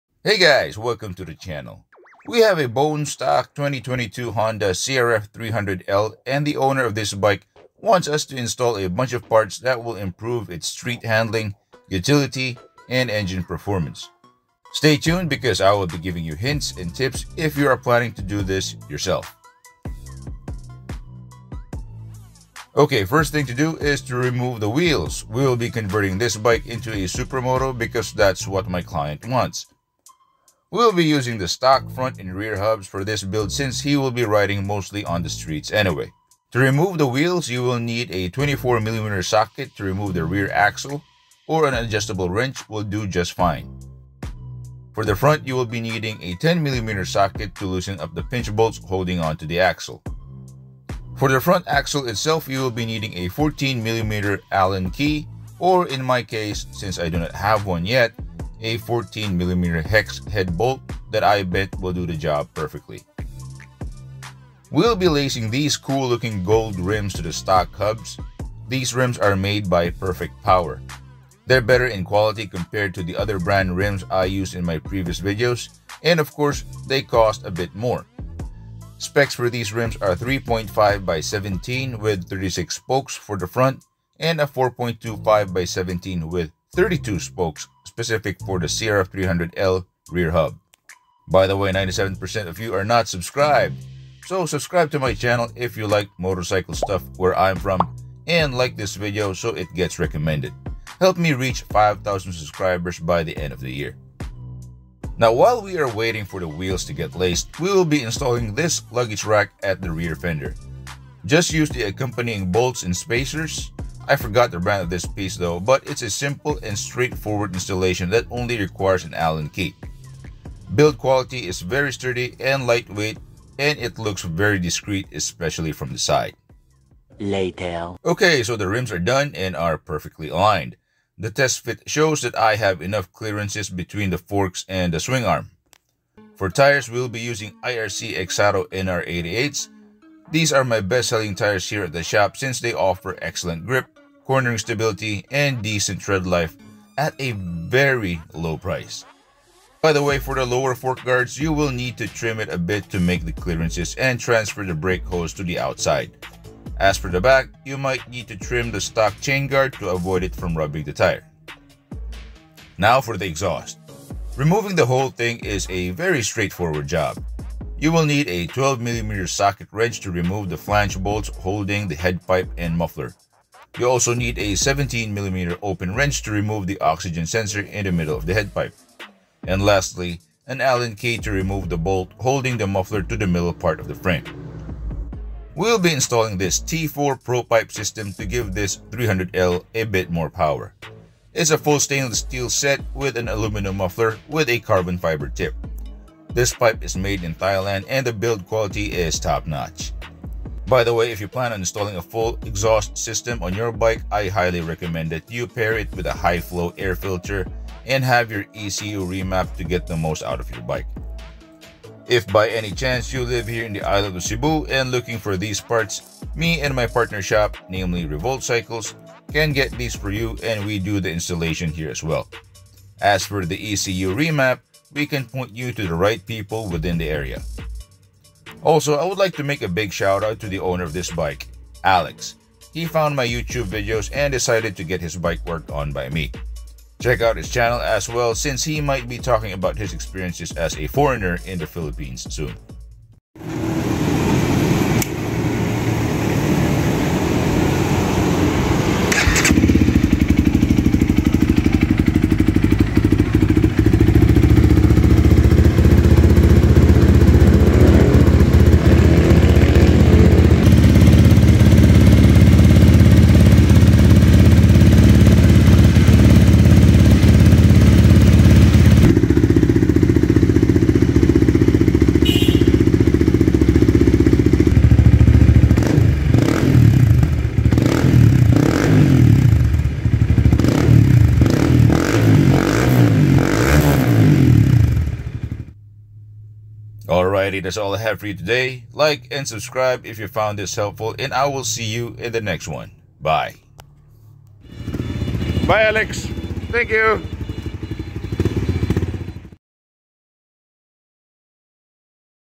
Honda CRF300L - Supermoto conversion and Exhaust install + luggage rack
So a customer brought in his 2022 Honda CRF300L for some upgrades. watch this if you plan on doing any of these to your bike.

Help me make more content! 🙂

How about a pre owned car or motorcycle?